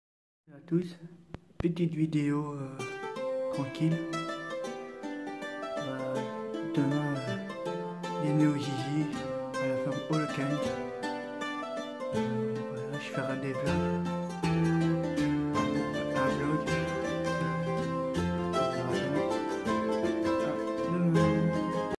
Annonce pour la sortie de demain
Dîner jj à la ferme holeken à linkebeek venez nombreux à demain les amis